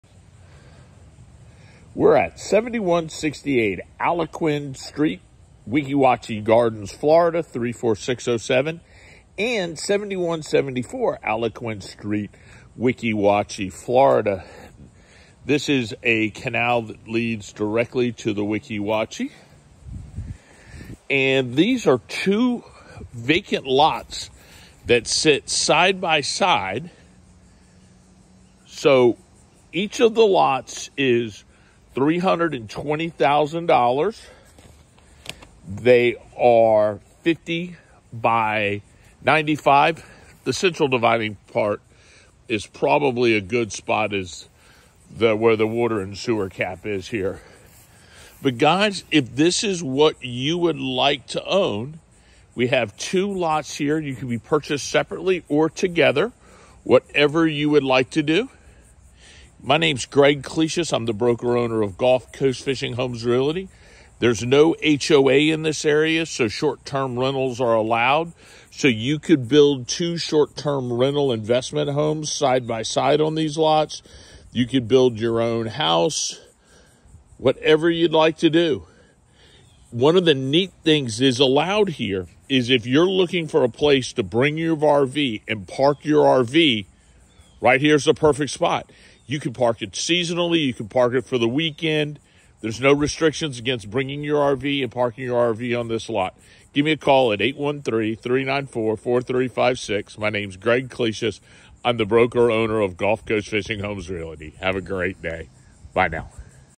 For Sale: 7168 Algonquin St Weeki Wachee FL 34607 for $320,000 – Weeki Wachee River Canal Lot
BUILD YOUR DREAM HOME ON A WEEKI WACHEE RIVER CANAL LOT WITH 50FT OF CANAL FRONTAGE.  This lot is 50 X 95 sits on a Weeki Wachee river canal that leads to the Weeki Wachee river so you can travel upriver to Weeki Wachee Springs State Park or downriver to the Gulf of Mexico. Watch manatees and dolphins swim in the canal behind your lot. NO HOA….YOU CAN PARK YOUR RV ON THE LOT AS LONG AS YOU LIKE…..SHORT TERM RENTALS ALLOWED…. SEWER AND WATER ARE AT THE STREET….. ZONE R1A….It is the buyer’s responsibility to perform any due diligence (including inspections) required or that they deem necessary. Street address needs to be verified with county. Build your own boat ramp… Use it for your RV/Camper on weekends or for the winter… then rent it when you are not using it…. Build your Weeki Wachee dream home….. Build a Short Term Rental investment home….  With this lot the opportunities are endless…. What is access to the Weeki Wachee River worth? Go to site. Gulf Coast Fishing Homes Realty.